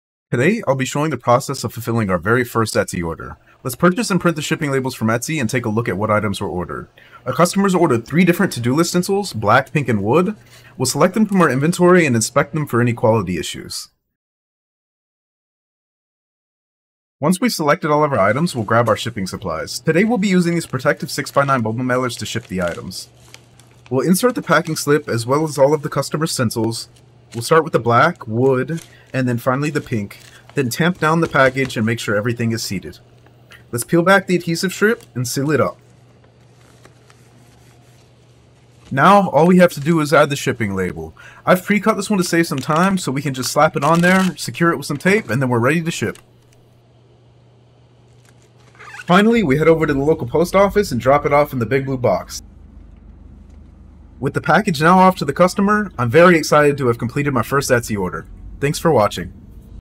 First Etsy Order
PrintPalaceXYZ.etsy.com

Today we're fulfilling our very first Etsy order, watch me package and ship the customers items and see how the entire Etsy process works.